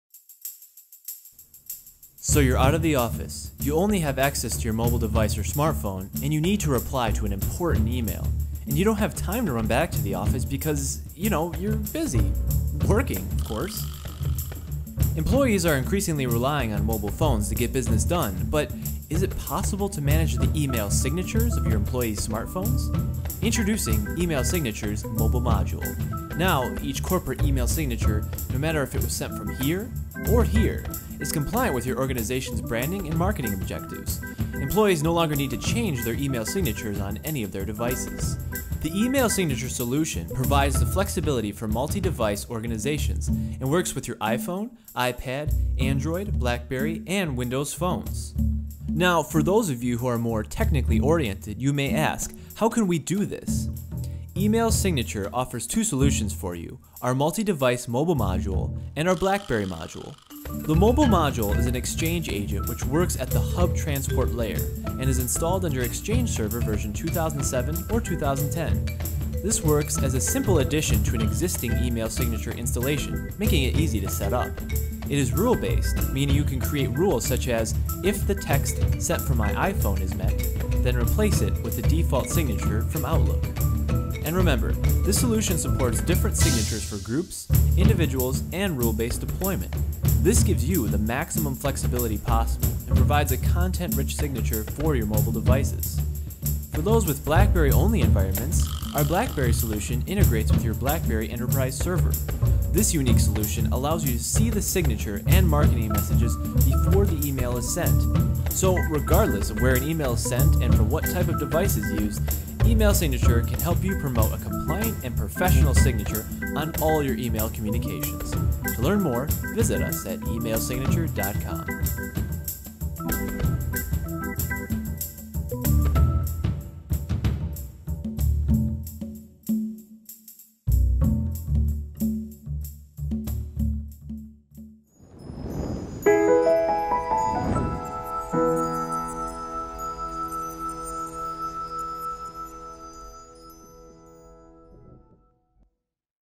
eMailSignature - Mobile & BlackBerry module
These days, sending emails isn't something that just happens on your desktop.
With the advent of smart phones, tablets and other devices, email has gone mobile!

Ensure your emails look great on BlackBerry, iPhone, Android and Windows Mobile phones.